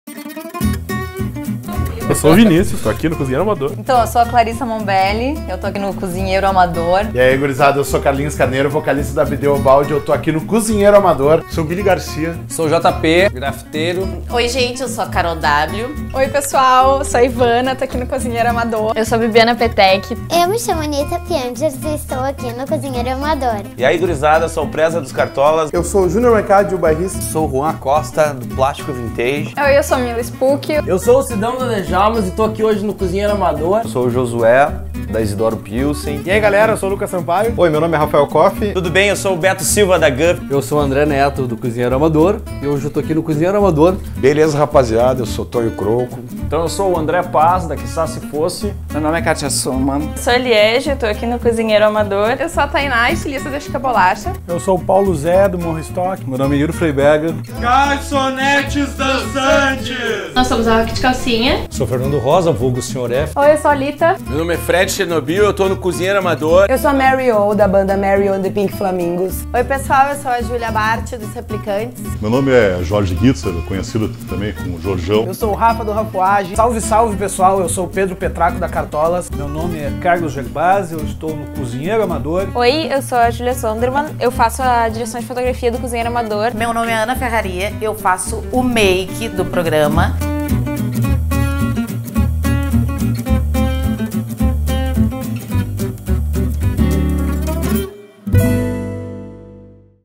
Retrospectiva 2014
2014 passou e depois de 39 episódios a gente sente que fez uma coisa muito bonita. Acreditamos que conectamos muitas pessoas legais. Que demos espaço para que artistas chegassem a novos públicos. Que apresentamos para vocês diferentes formas de cultura. Isso sem contar as receitas bacanas que todos nós aprendemos. Mas o que sempre marcava a gente depois de cada gravação era esse sentimento de que estávamos fazendo algo bonito e que a cada noite a gente entrava na cozinha e na vida de alguém e raramente essa pessoa saía da nossa vida.

Obrigado cozinheiros.
Obrigado equipe.
Obrigado audiência.
2014 foi muito bom.
Que 2015 seja melhor ainda.

André Neto: apresentador e diretor de arte.
Adelino Bilhalva: diretor, produtor, diretor sonoro e montador.
Julia Sondermann: direção de fotografia e câmera.
Ana Cláudia Ferrary: making of e make up.